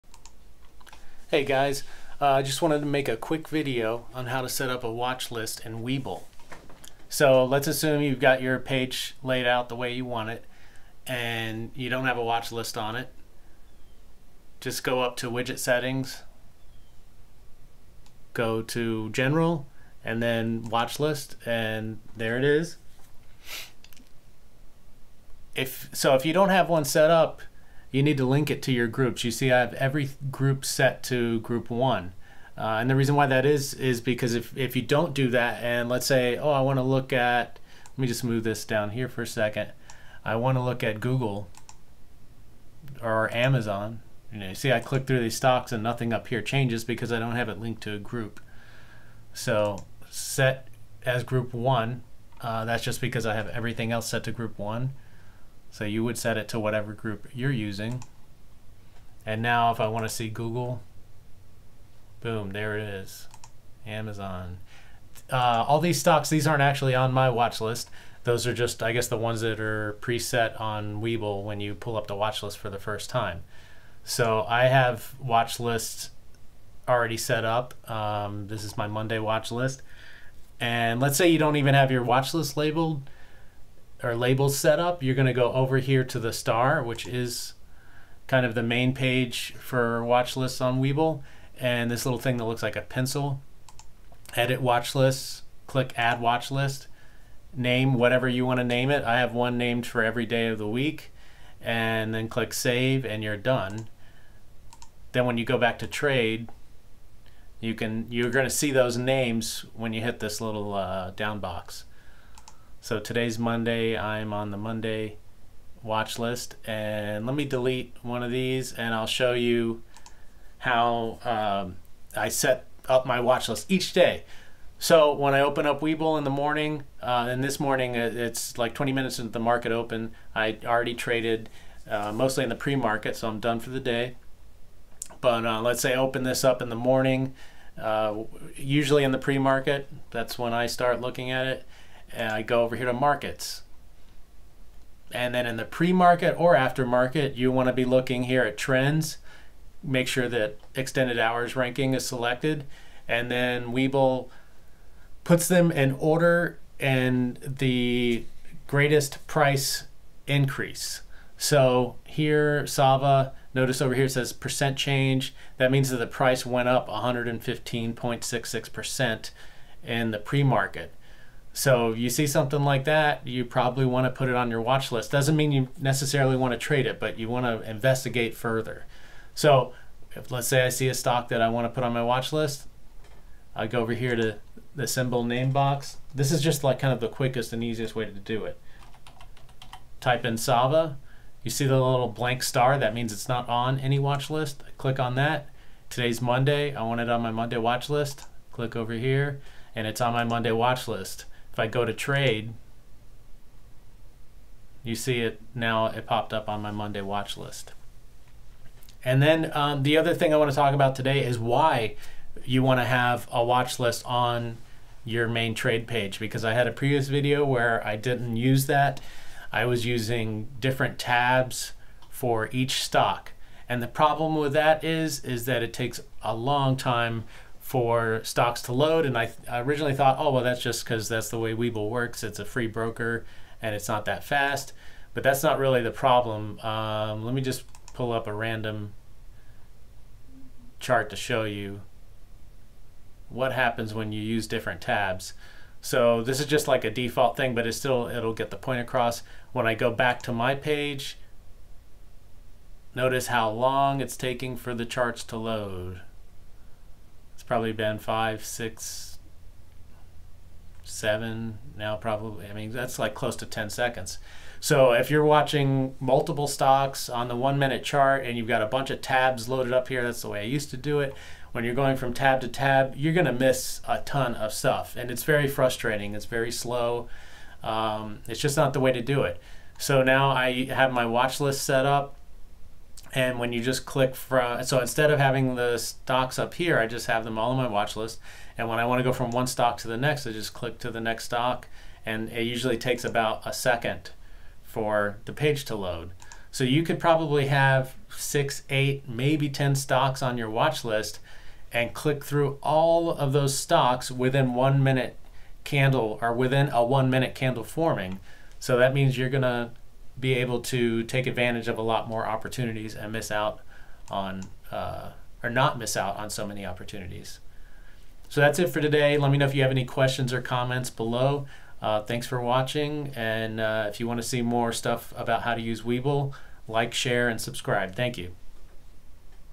HOW TO DAY TRADE WITH WEBULL | WATCH LISTS
How to setup a watch list of good stocks to trade on Webull.

If you would like to open an account with Webull and get a free stock worth up to $1,600.00